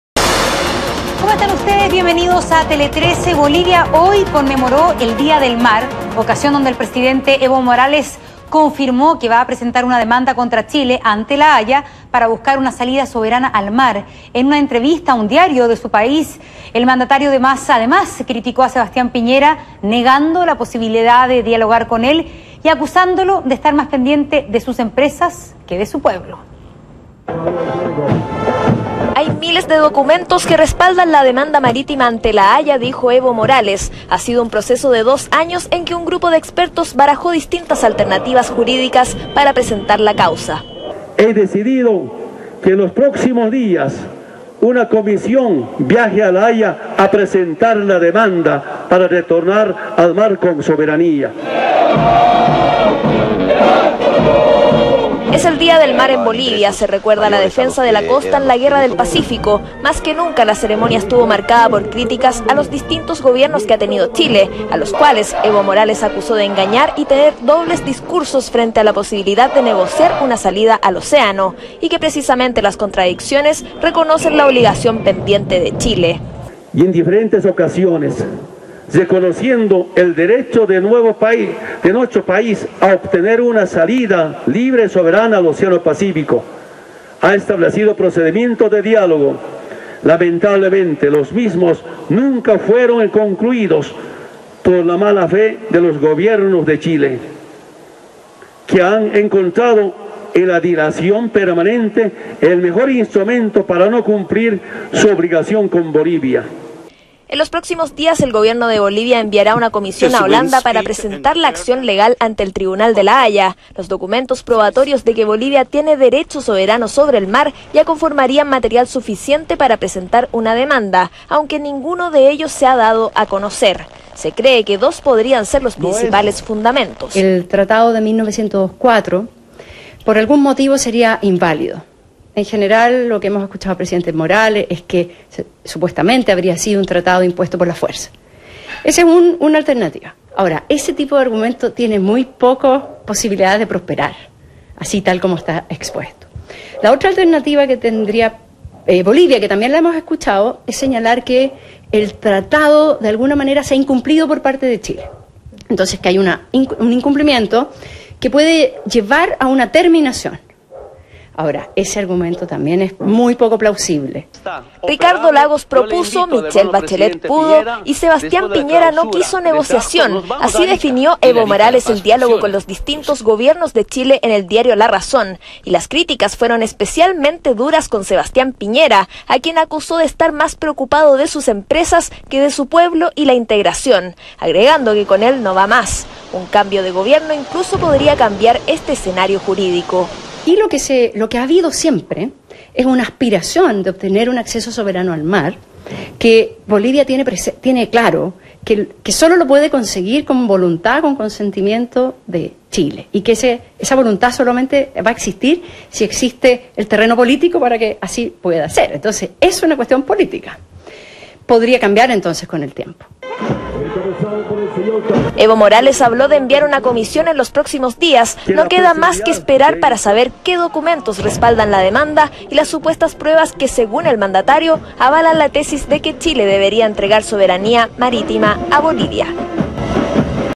Bolivia demandará a Chile ante La Haya.
Un enfático Evo Morales confirmó la demanda contra Chile ante la Haya. El anuncio se hace justo en la jornada en que Bolivia celebra el "Día del Mar", y después de que este viernes el Congreso lo habilitara para ello.